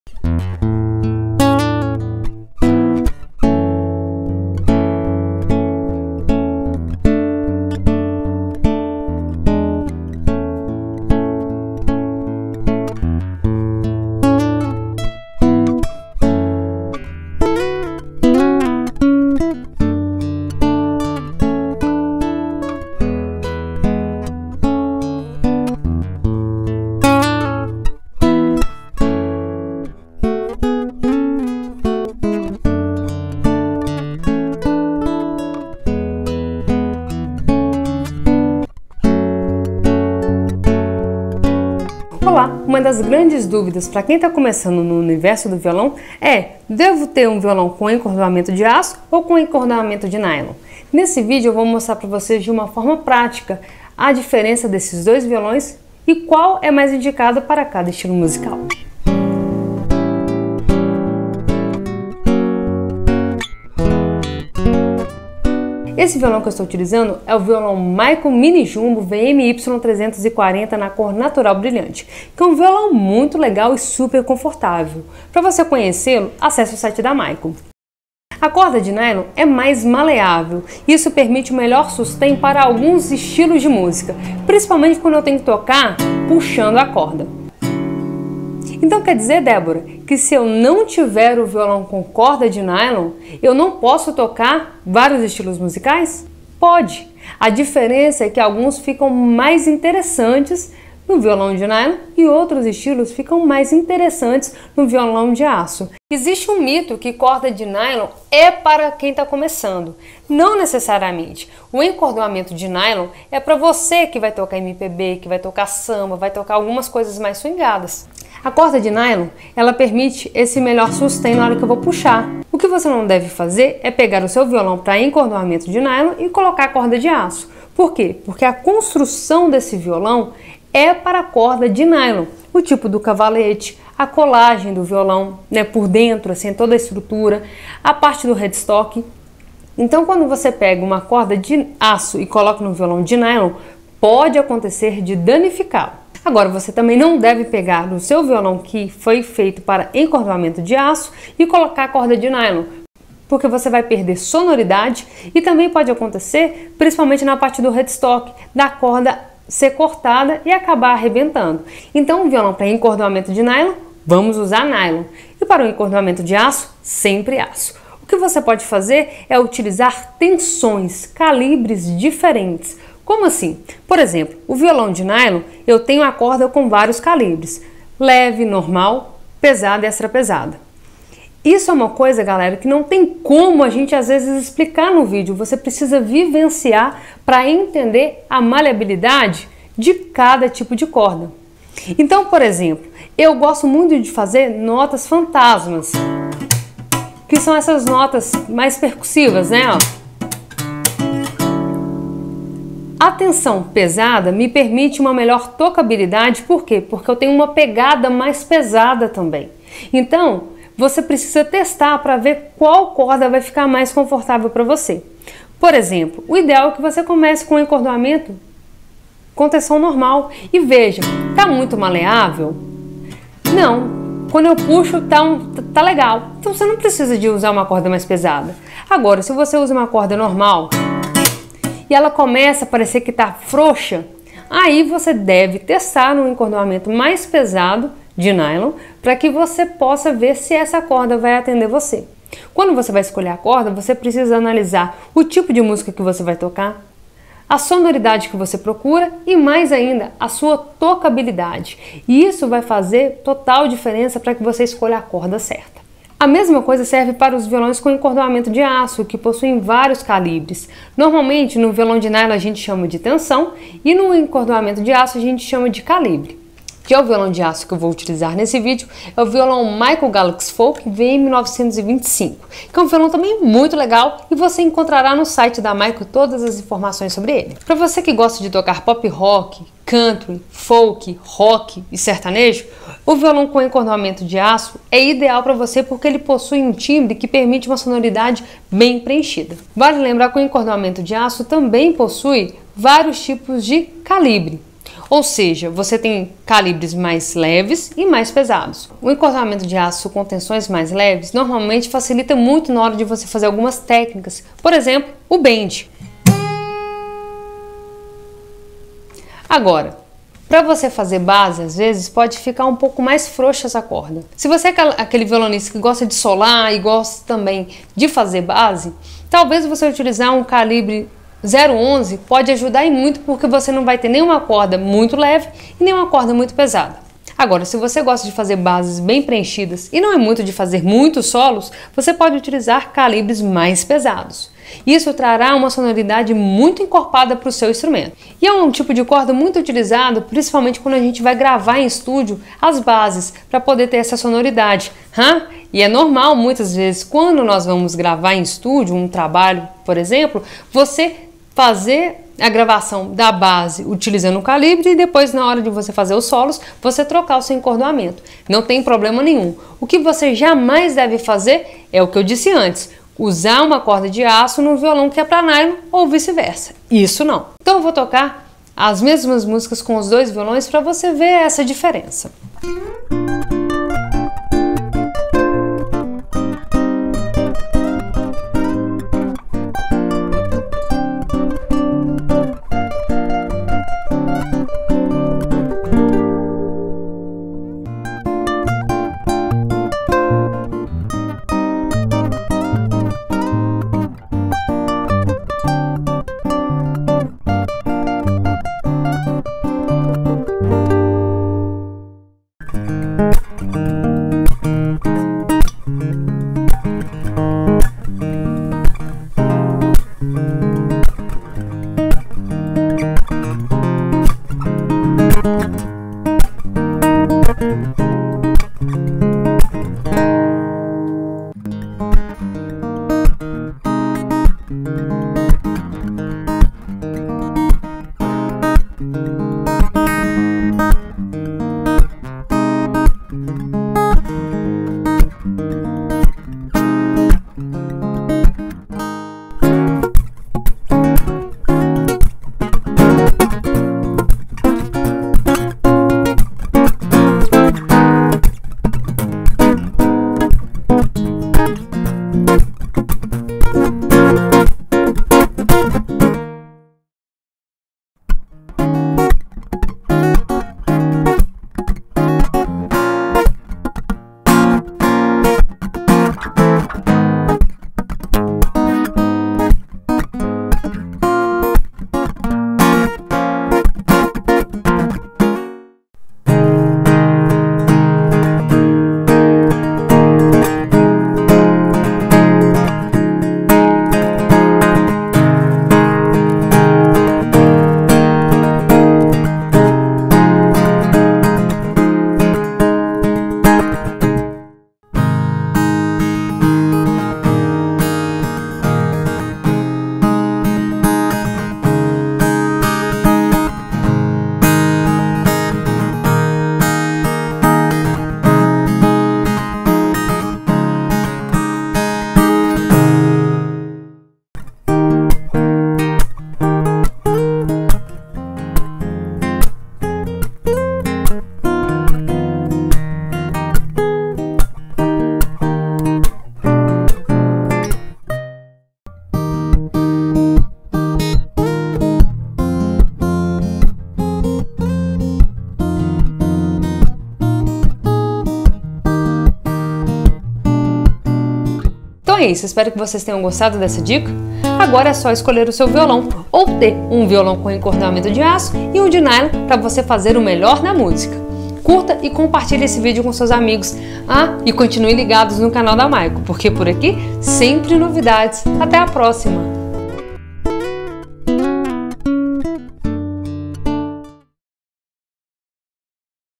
Violão: Corda de Aço ou Nylon? Entenda a diferença
No mundo violonístico é sempre comum a dúvida sobre qual tipo de encordoamento usar. Nesse vídeo nossa artista Débora Ildêncio fez esse vídeo falando sobre esse universo e ela exemplifica as mesmas músicas com o violão com encordoamento de aço e nylon para que você consiga observar as nuances de cada instrumento.

Aperte o play e confira

Créditos:
Captação de imagem e áudio: Débora Ildêncio
Edição audiovisual: Débora  Ildêncio
Criação de caracteres: Verlane Batista